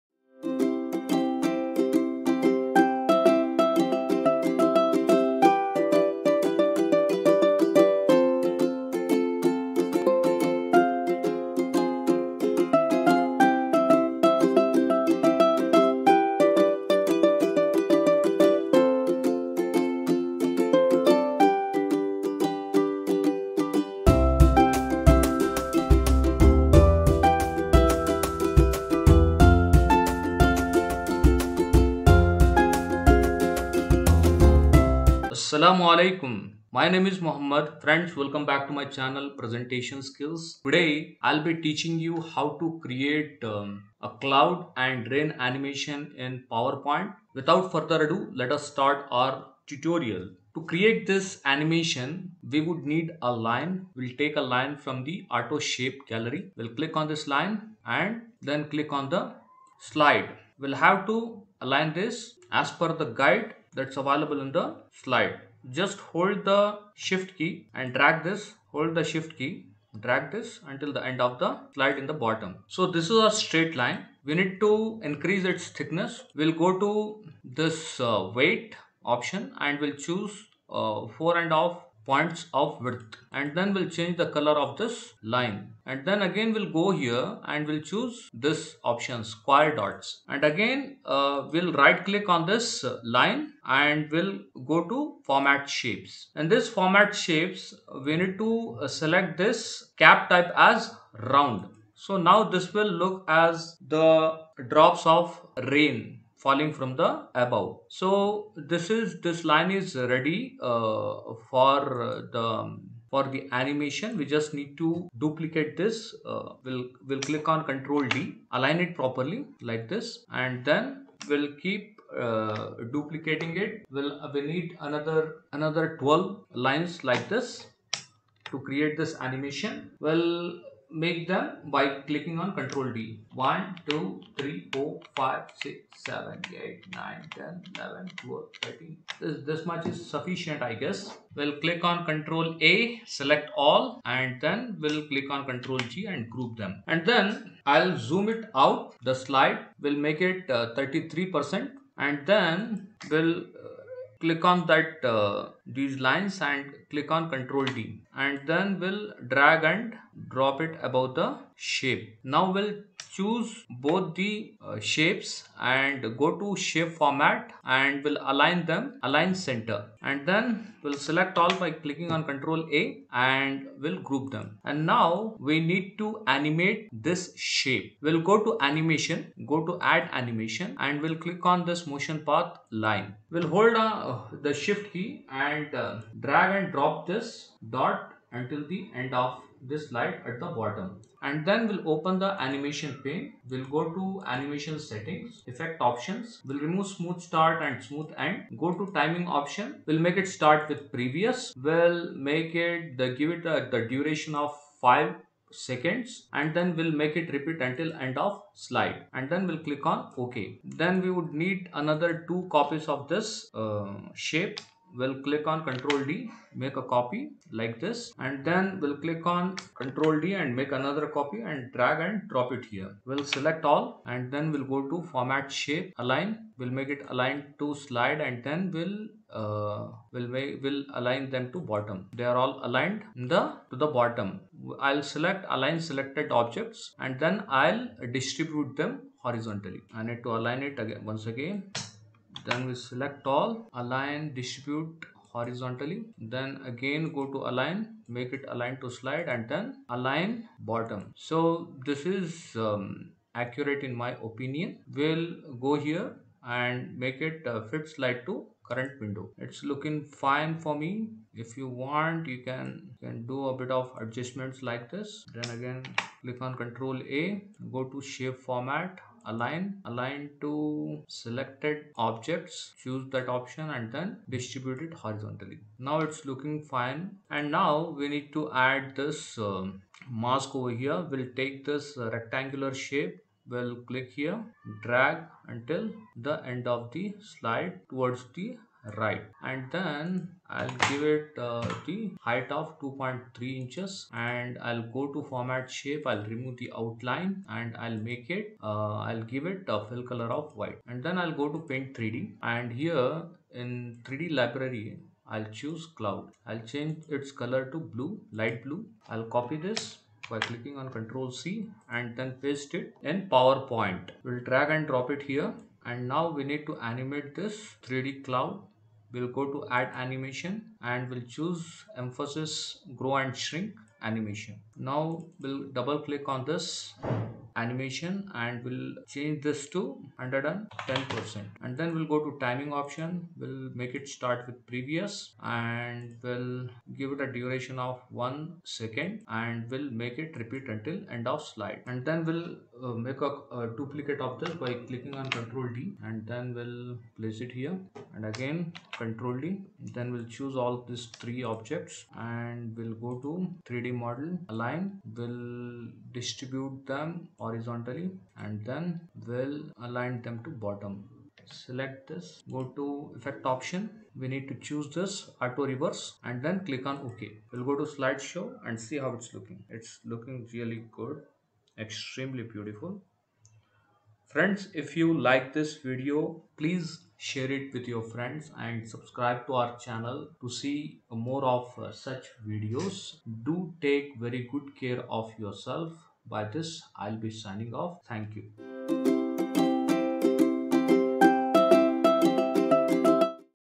How to create 3D Cloud and Rain Animation in PowerPoint | PowerPoint me badal aur barish kese banaye
Hi One and all,

Regarding this video: There you have the best best video on 3D Cloud and Rain Animation in PowerPoint.

kindly share it with as many friends of yours as possible. It will keep my motivation level high and encourage me to create more such content for you in future.